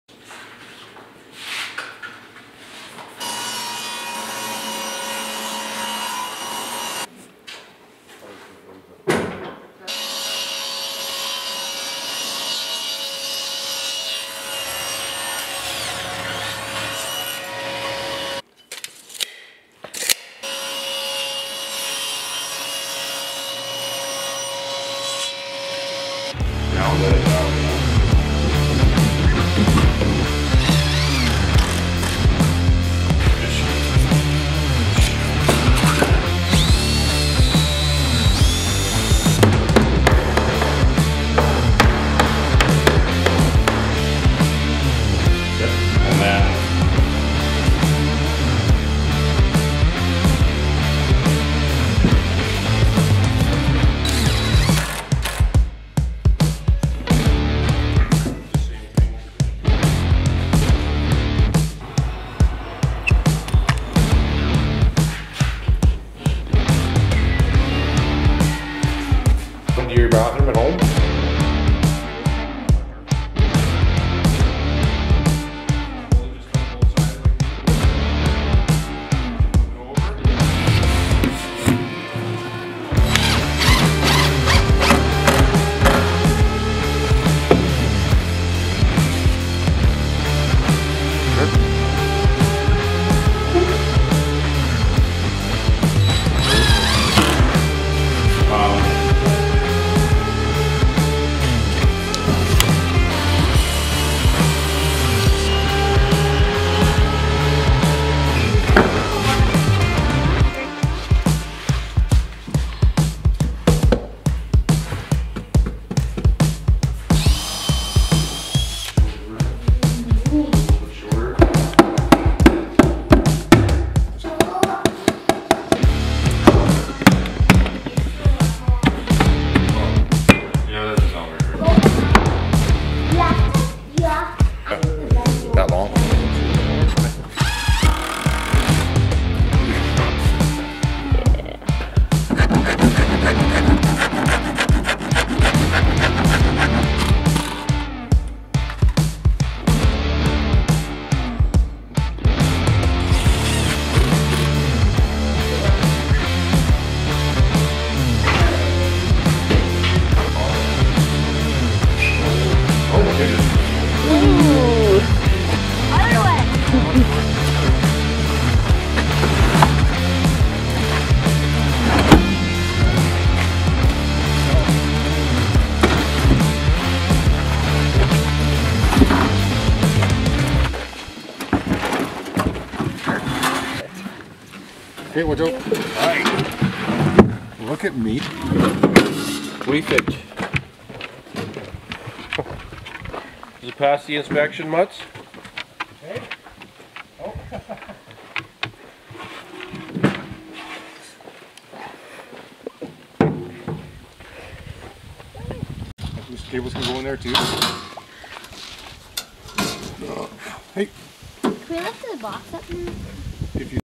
Making A Pick Up Truck Tool Box
In this Video The Cussing Carpenter makes a tool box for Pick-Up Trucks.